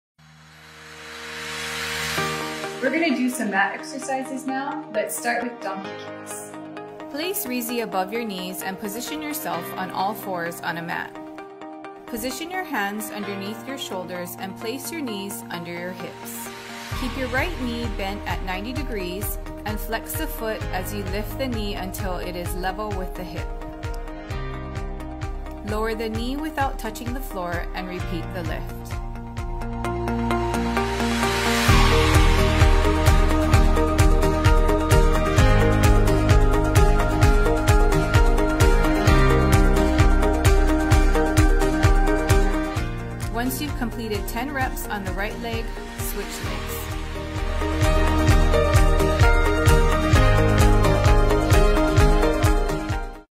Donkey Kicks with Reezy Bands
Procedure

1) Position yourself on all fours on a mat.
2) Position your hands underneath your shoulders and place your knees under your hips.
3) Keep your right knee bent at 90 degrees and flex the foot as you lift the knee until it is level with the hip.
4) Lower the knee without touching the floor and repeat the lift.
5) Once you’ve completed 10 reps on the right leg, switch legs.

Muscles Worked: Primary muscle group (s)
• Glutes and hip flexors

Secondary
• Abs